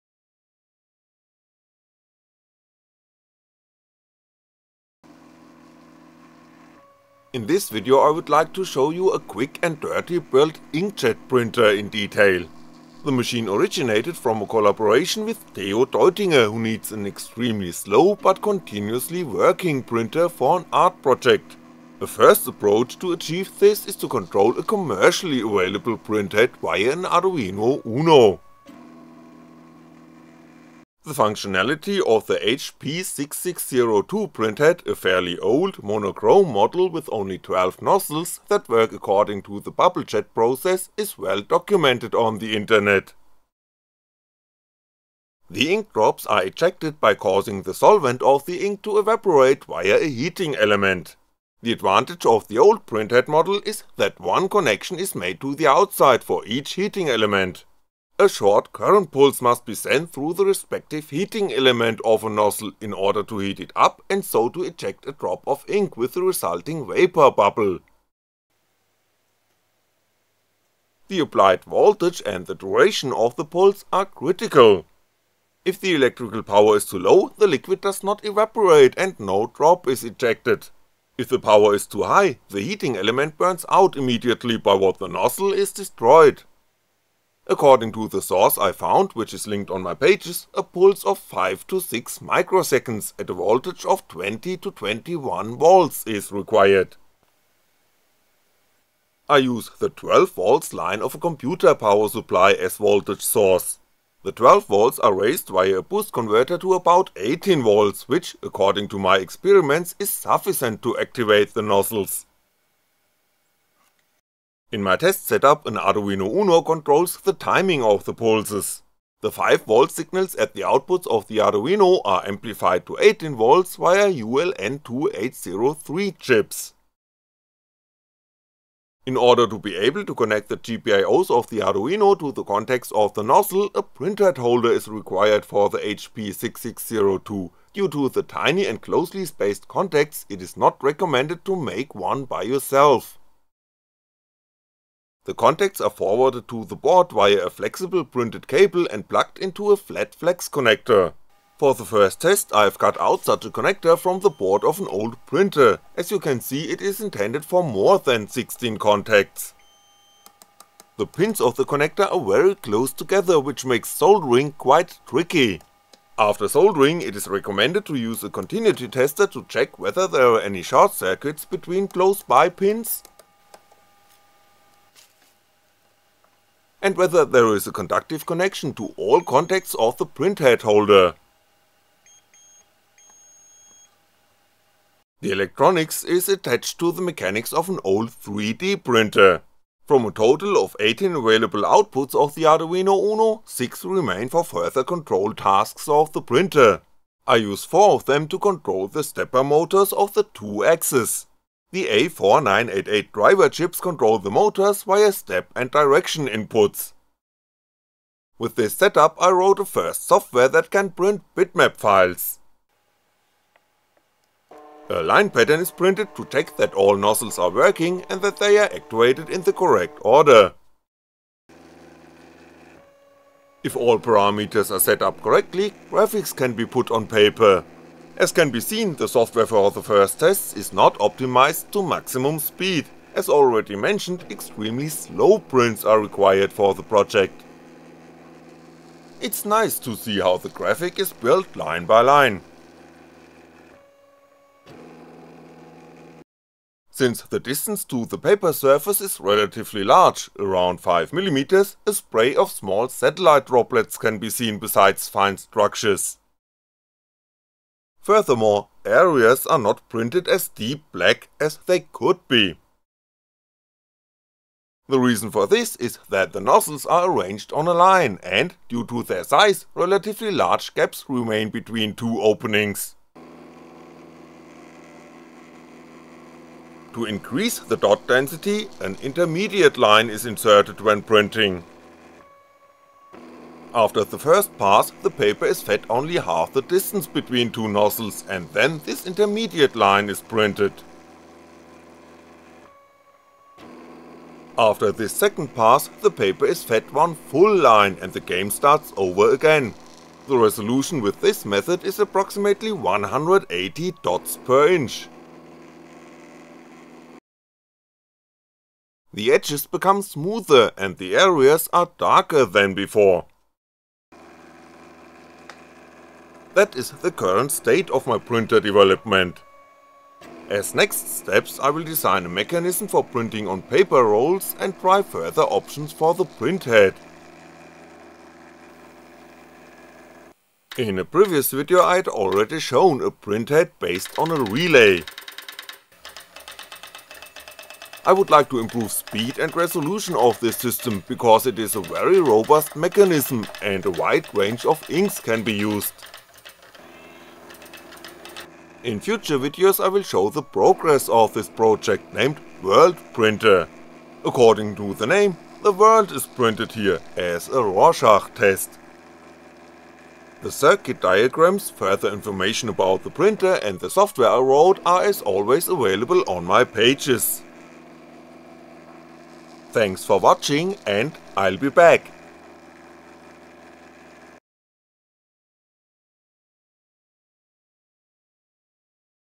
Controlling an HP6602 printhead with an Arduino UNO
Table Of Contents:
00:00 Introduction
00:20 HP6602 printhead
01:38 Electronics
03:33 Test prints
04:35 Double dot density
05:15 Conclusion